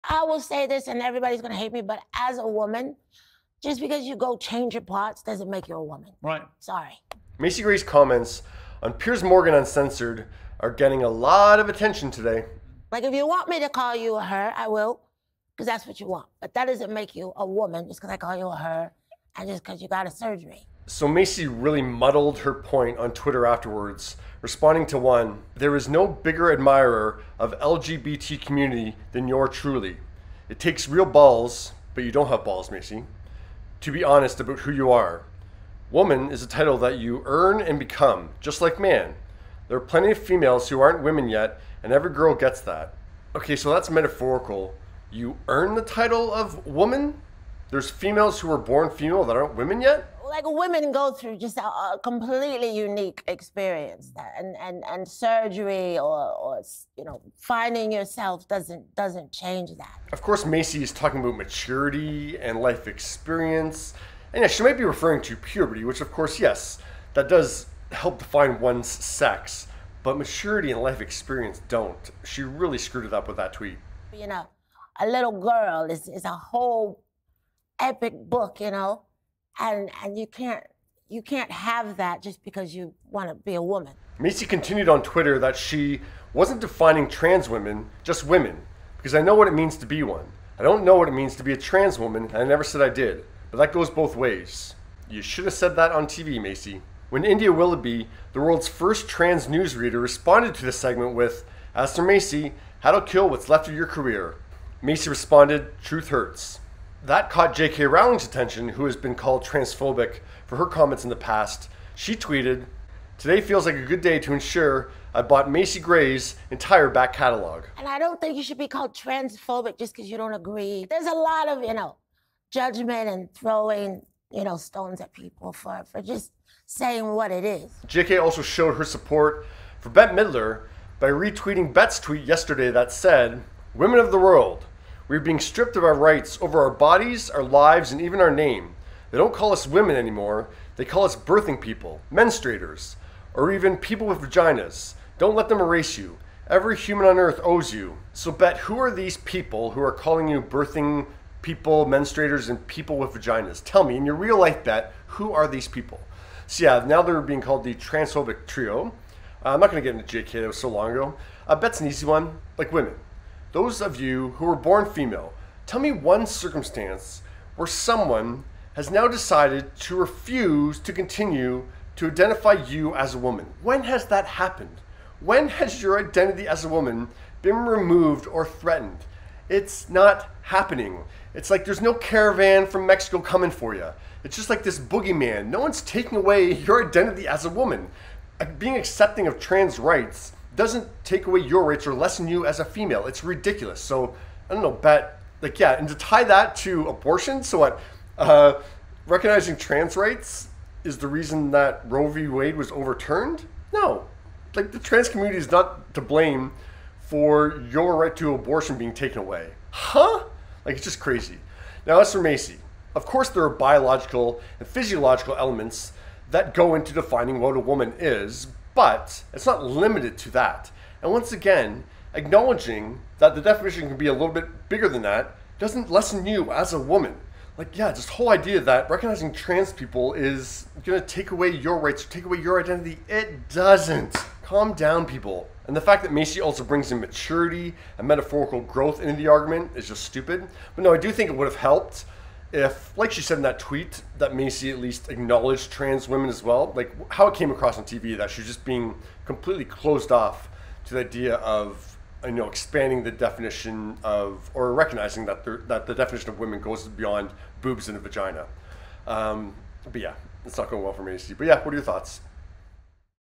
JK Rowling Supports Macy Gray, Bette Midler ‘Woman’ Comments
Graeme O'Neil takes a look at Macy Gray defining "woman" on "Piers Morgan Uncensored", and Bette Midler's tweet to women that are being called transphobic but got the support of J.K. Rowling.